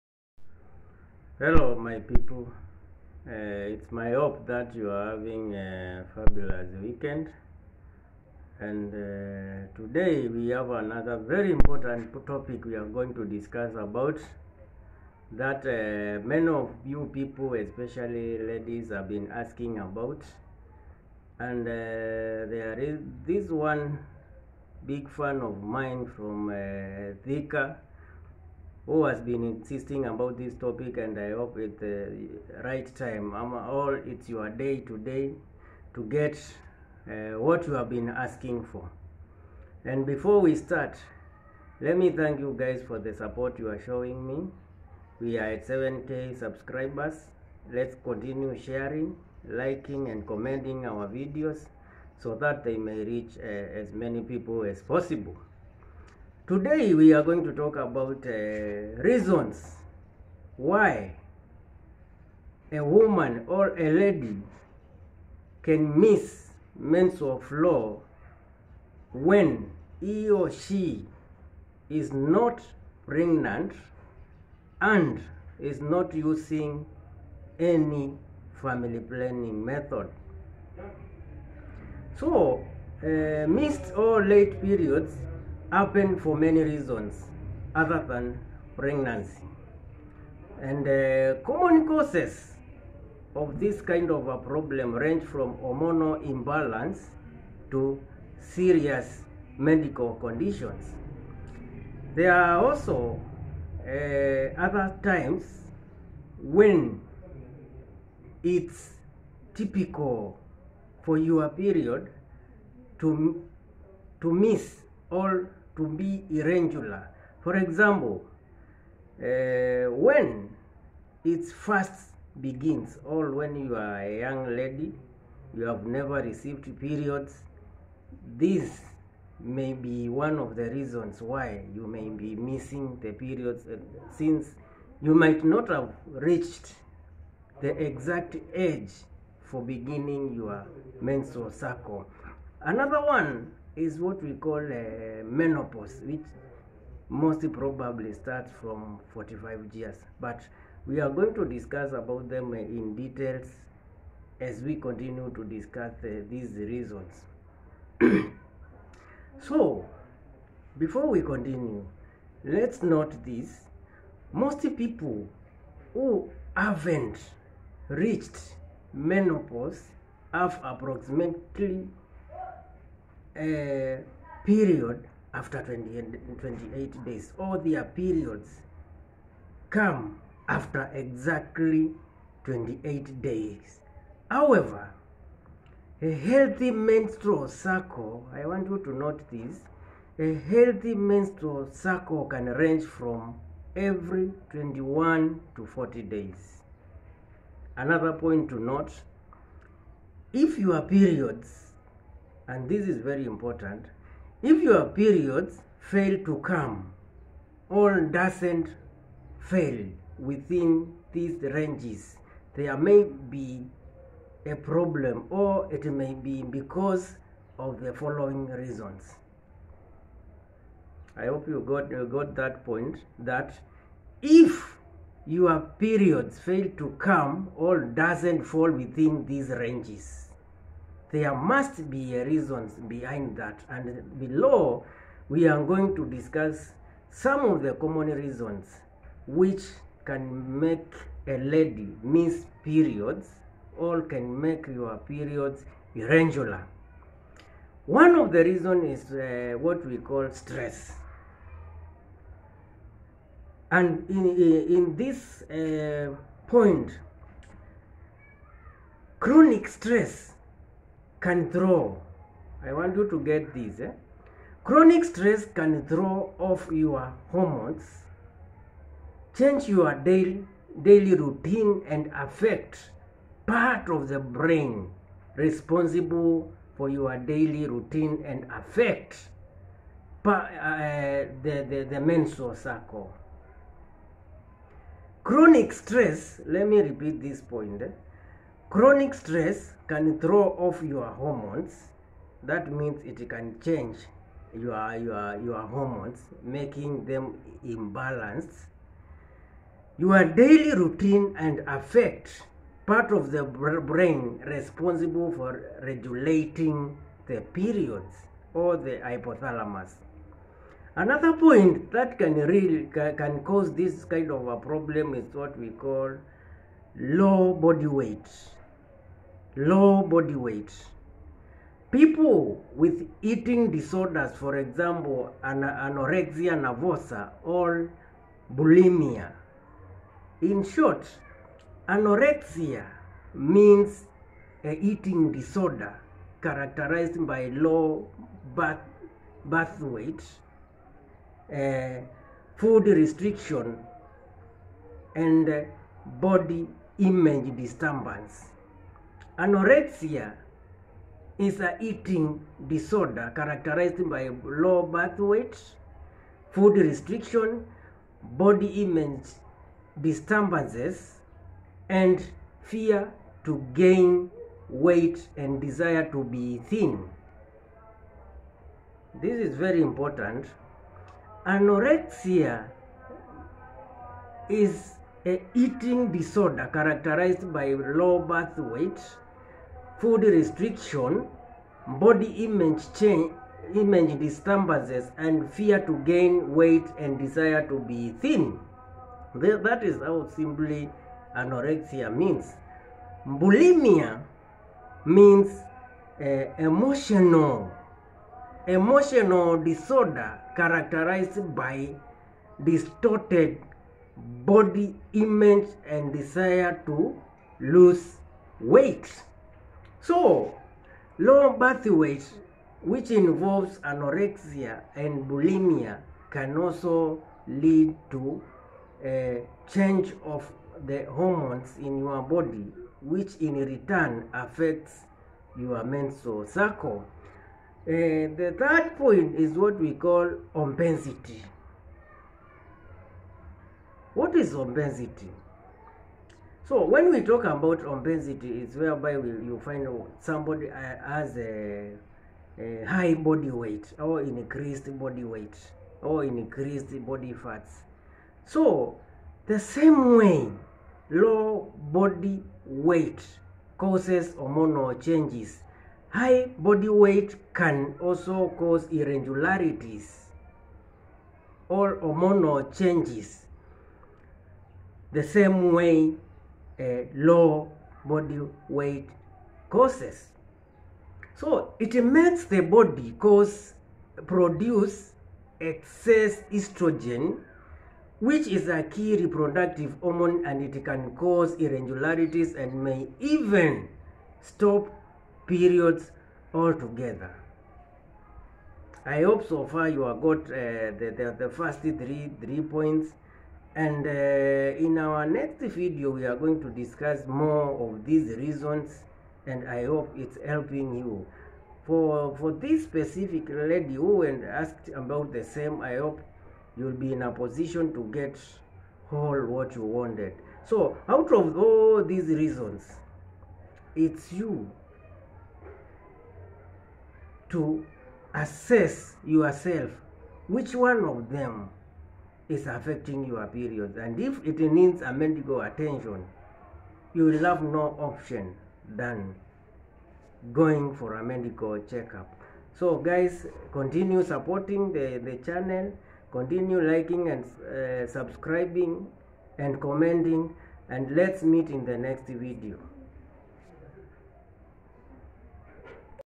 @hikemedicalservices; WHY A LADY CAN MISS MENSTRUAL FLOW WHEN NOT PREGNANT & NO CONTRACEPTIVES
why a lady can miss menstrual flow when not pregnant and no contraceptives